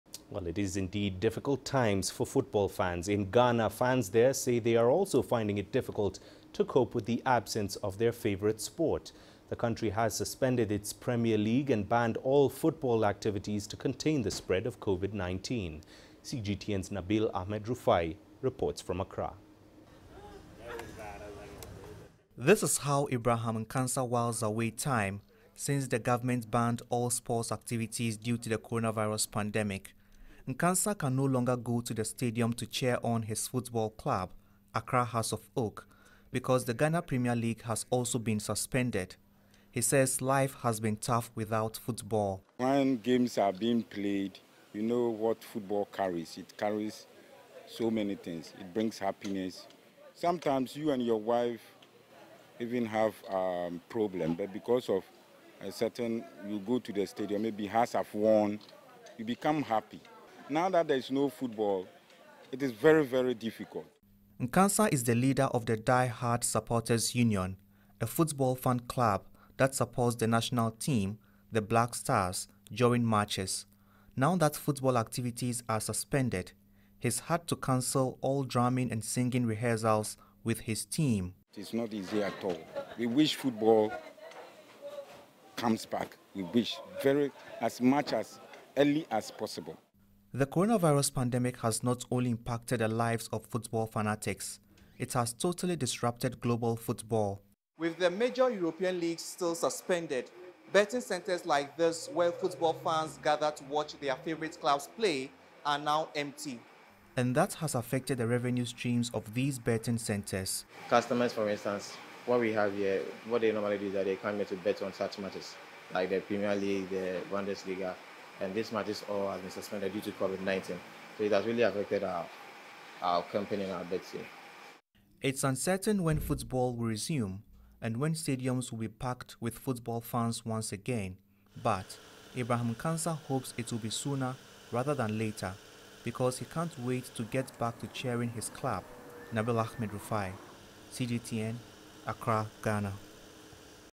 Ghana die hard supporters lament lack of football
In Ghana, football fans say they are finding it difficult to cope with the absence of their favourite sport. The country has suspended its premier league and banned all football activities to contain the spread of COVID-19. CGTN's Nabil Ahmed Rufai reports from Accra.